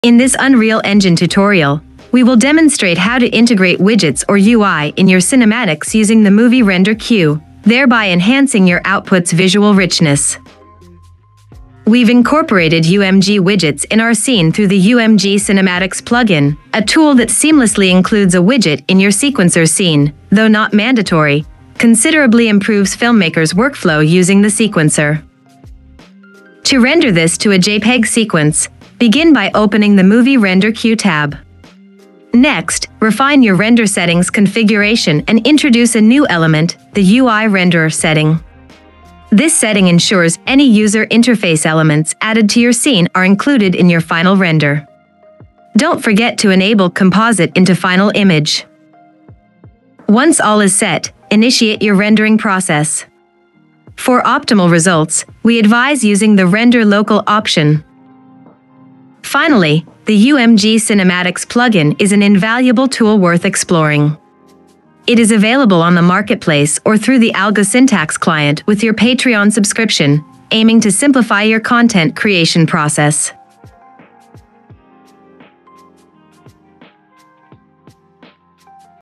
UE5 Movie Render Queue: How to Render UMG/UI
Unleash the power of the Movie Render Queue in Unreal Engine with our tutorial on integrating UI widgets into your cinematics. Learn about UMG Widgets, configuring render settings, and effective rendering practices. Improve your workflow with the UMG Cinematics Plugin, available at